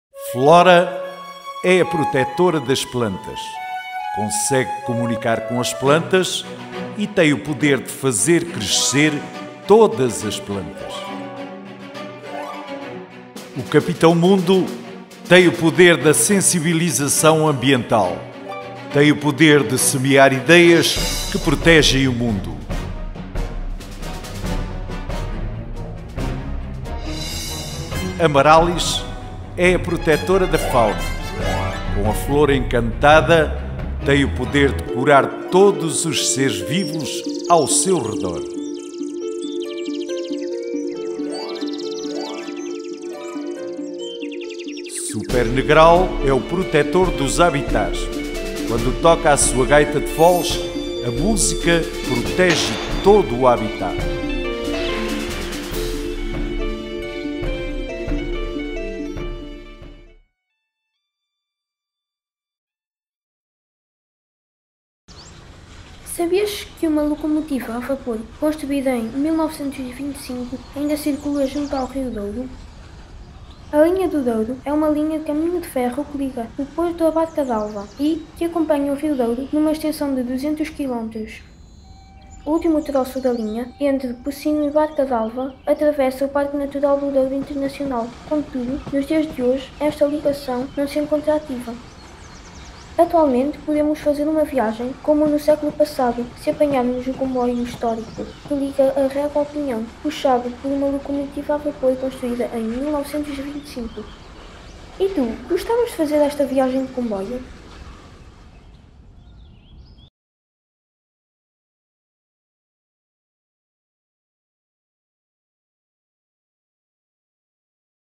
FNT04E16 - PN do Douro Internacional: Locomotiva a vapor
Sabias que uma locomotiva a vapor construída em 1925 ainda circula junto ao rio Douro?